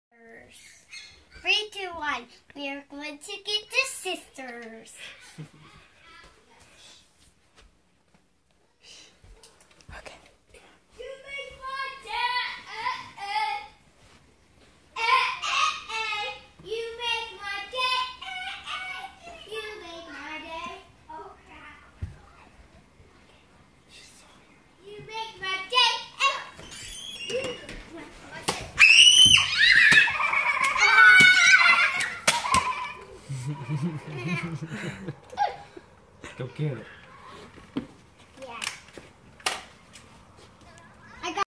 Nerf gun surprise attack on the sisters!
Nerf gun sneak up attack on the sisters